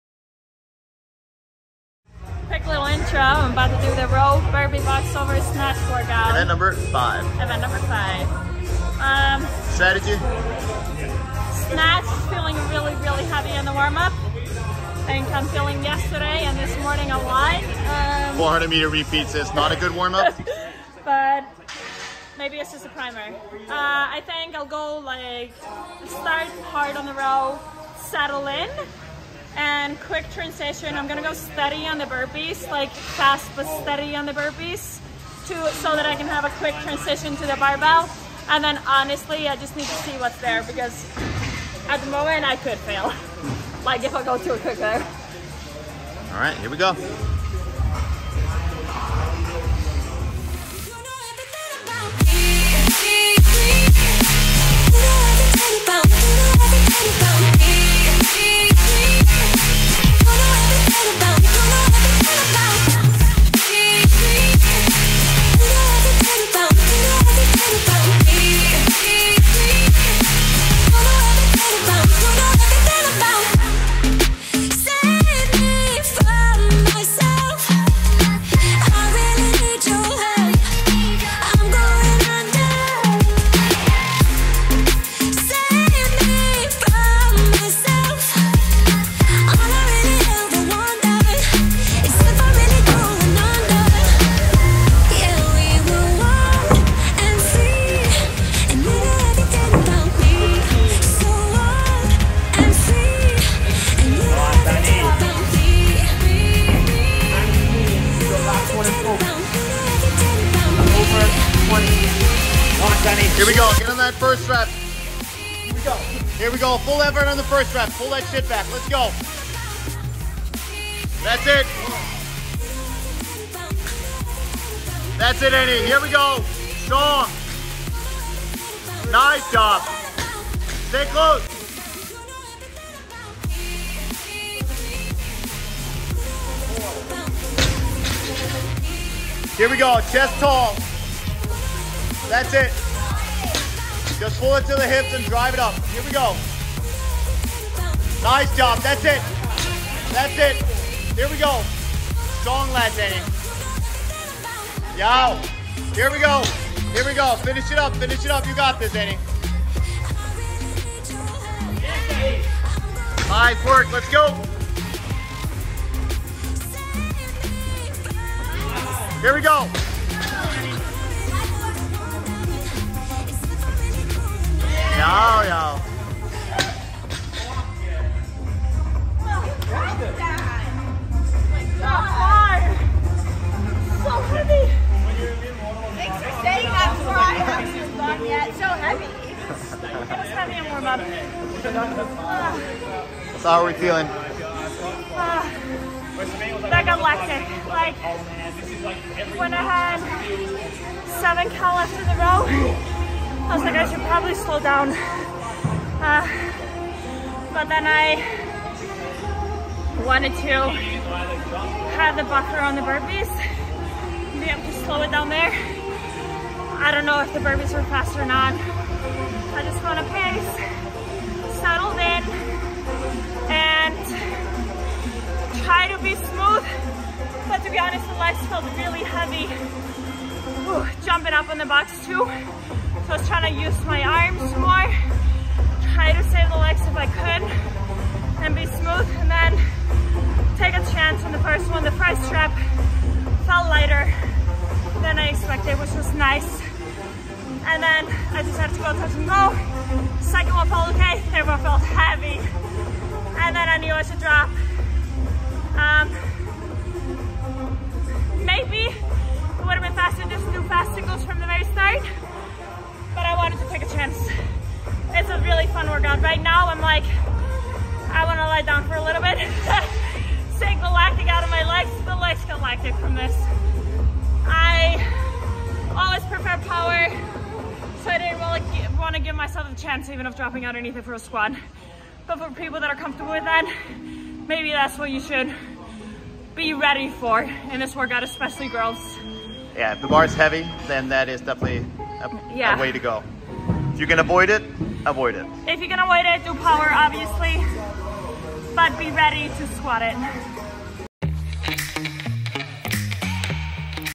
CrossFit Games Quarterfinals event 5
30 cal row
20 BBJO 24-20”
10 snatches 185/135